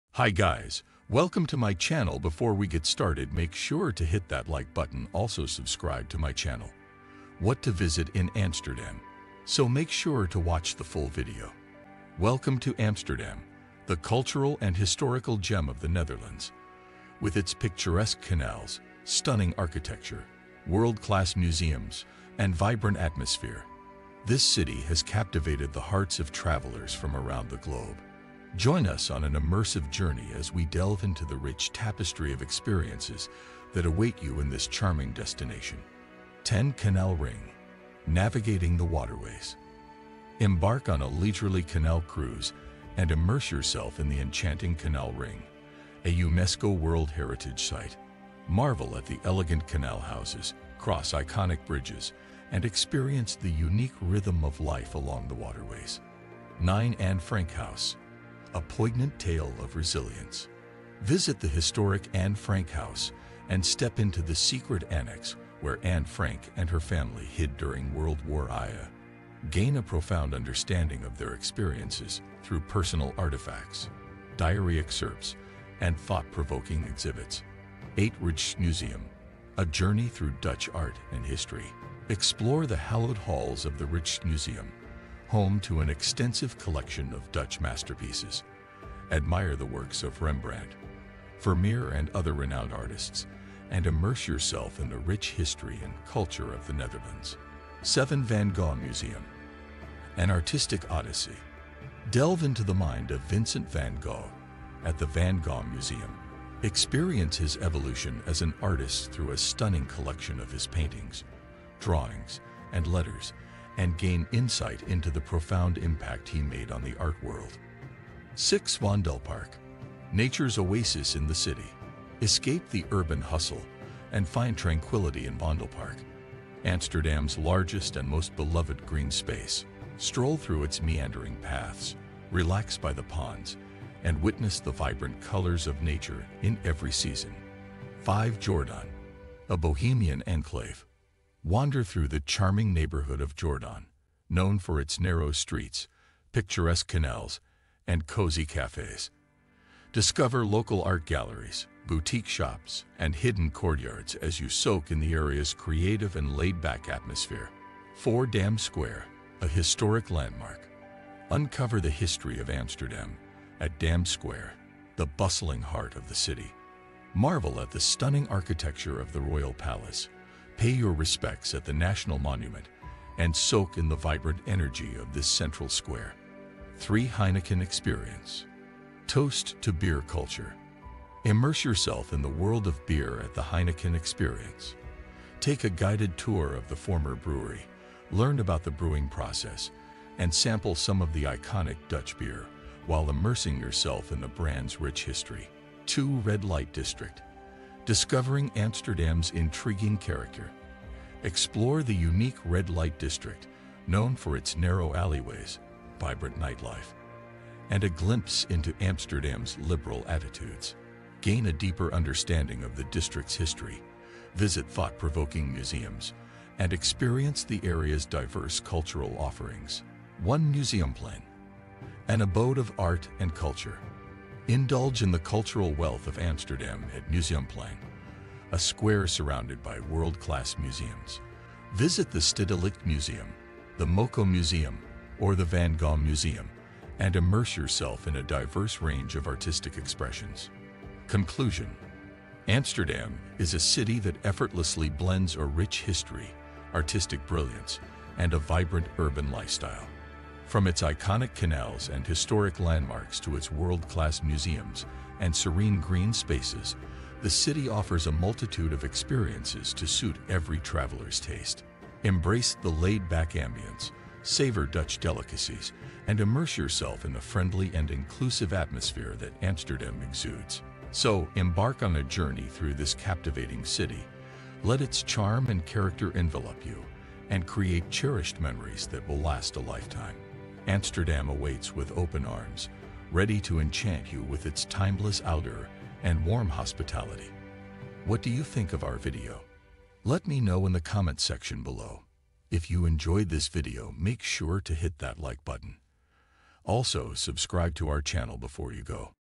Amsterdam Travel Guide: What To Visit In 2024
What to visit in Amsterdam

Welcome to Amsterdam, a cultural gem with picturesque canals, world-class museums, and charming destinations. Explore its rich history and vibrant atmosphere.

[00:00] Amsterdam - the cultural and historical Gem of the Netherlands

[00:37] Explore the enchanting Amsterdam canals and visit The Anne Frank House

[01:17] Explore Dutch art and history at Rich Museum and Van Gogh Museum

[01:54] Gain insight into the profound impact on the art world

[02:32] Explore the shops and hidden courtyards in Amsterdam's creative atmosphere.

[03:09] Immerse yourself in Amsterdam's red light district and explore its intriguing character

[03:49] Amsterdam is a city that effortlessly blends a rich history, artistic brilliance, and a vibrant urban lifestyle.

[04:27] Amsterdam captivates with its charm and character.

[00:00 Amsterdam - the cultural and historical Gem of the Netherlands
- Picturesque canals and stunning architecture
- World-class museums and vibrant atmosphere

[00:37] Explore the enchanting Amsterdam canals and visit the Anne Frank House
- Embark on a canal cruise to experience the unique rhythm of life along the waterways
- Marvel at the elegant canal houses and iconic bridges
- Step into the Secret Annex at the historic Anne Frank House to gain a profound understanding of their experiences

[01:17] Explore Dutch art and history at Rich Museum and Van Gogh Museum
- Rich Museum offers an extensive collection of Dutch masterpieces including works by Rembrandt and Vermeer
- Vanguard Museum showcases the evolution of Vincent Van Gogh as an artist through a stunning collection

[01:54] Gain insight into the profound impact on the art world
- Explore Wandelpark Nature's Oasis in the city
- Experience tranquility in Amsterdam's largest green space
- Stroll through the meandering paths and witness vibrant colors of nature
- Wander through the charming neighborhood of Jordan known for its narrow streets and picturesque canals
- Discover local art galleries

[02:32] Explore the shops and hidden courtyards in Amsterdam's creative atmosphere.
- The historic Dam Square is a must-visit with its stunning architecture and vibrant energy.
- Experience the beer culture at the Heineken experience by taking a guided tour of the former brewery.

[03:09] Immerse yourself in Amsterdam's red light district and explore its intriguing character
- Sample some iconic Dutch beer and learn about the Brand's rich history
- Experience the vibrant nightlife and narrow alleyways
- Visit thought-provoking museums and learn about the district's history
- Indulge in the cultural wealth of Amsterdam at the One Museum

[03:49] Amsterdam is a city that effortlessly blends a rich history, artistic brilliance, and a vibrant urban lifestyle.
- Visit the Stedelijk, Moko Museum, or the Vanguard Museum to immerse yourself in a diverse range of artistic expressions.
- Embrace the laid-back ambience, savor Dutch delicacies, and explore the multitude of experiences Amsterdam offers.

[04:27]( Amsterdam captivates with its charm and character.
- The city exudes a timeless outer and warm hospitality.
- Awaits with open arms to enchant visitors with its captivating atmosphere.